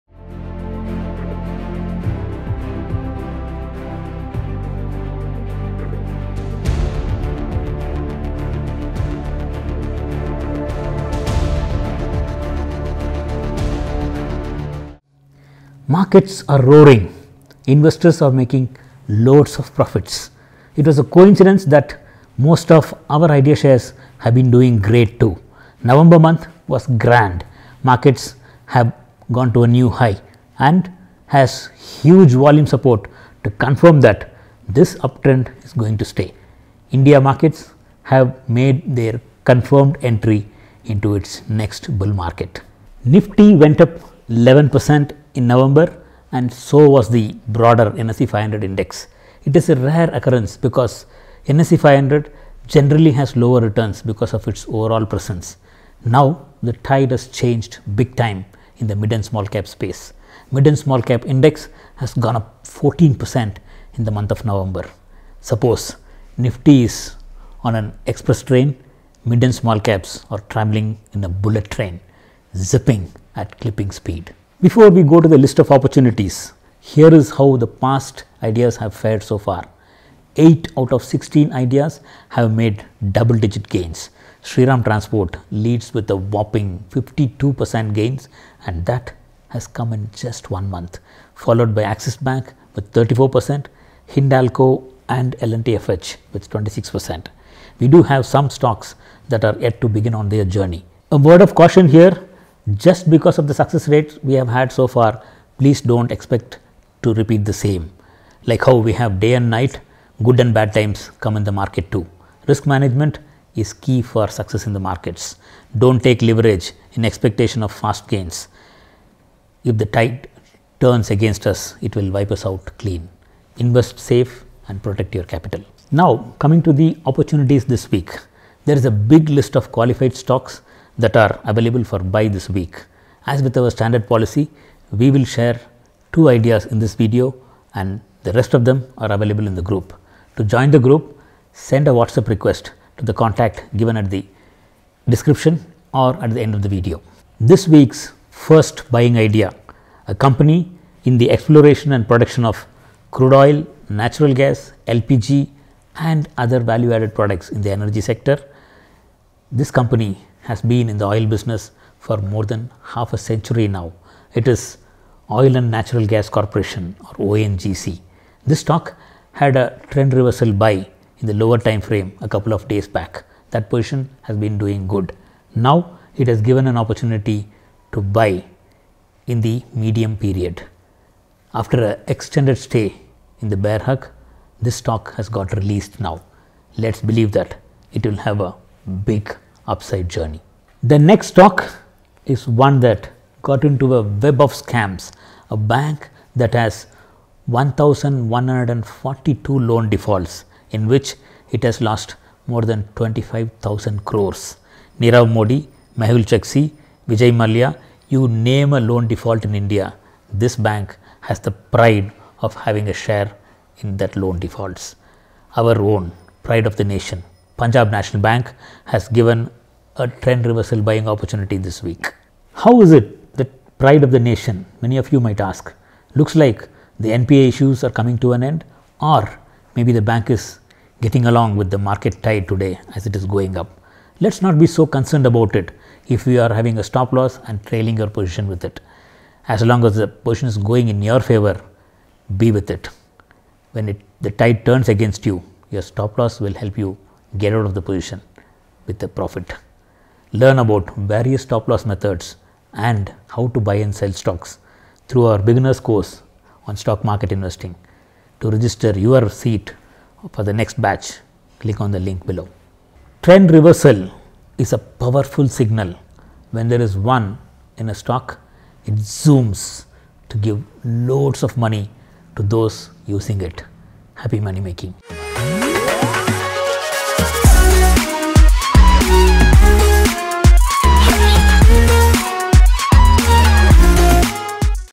Ready to Buy - ONGC and PNB
ONGC and PNB having a trend reversal buying opportunity as the markets get into a strong bull run.
Open your own Trading A/c with Zerodha:-
Want a stock to be reviewed, we might have done it.